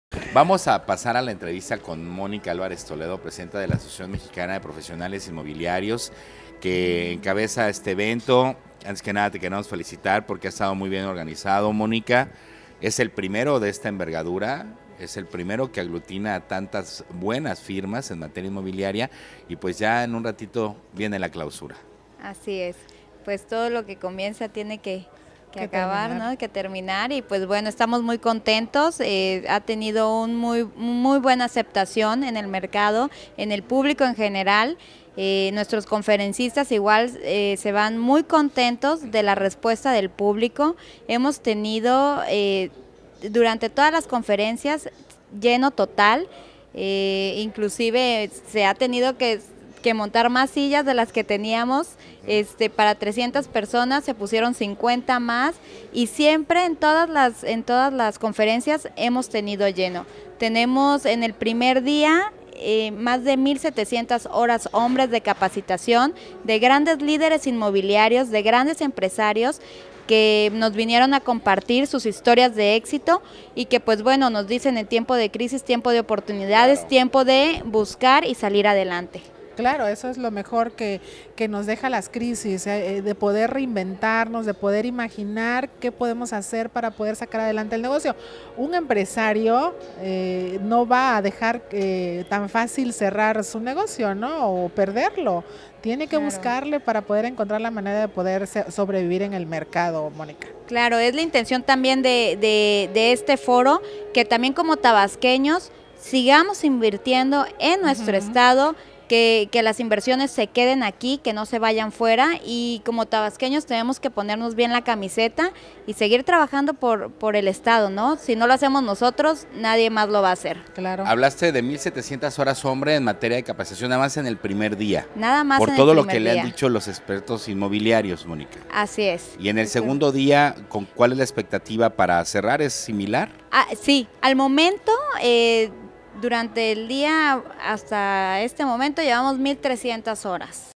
Concluye con éxito el 1er Foro Inmobiliario AMPI 2016, Entrevista a Mónica Álvarez Toledo
Concluye con éxito el 1er Foro Inmobiliario AMPI 2016 - Entrevista a Mónica Álvarez Toledo, Presidenta de la AMPI Tabasco.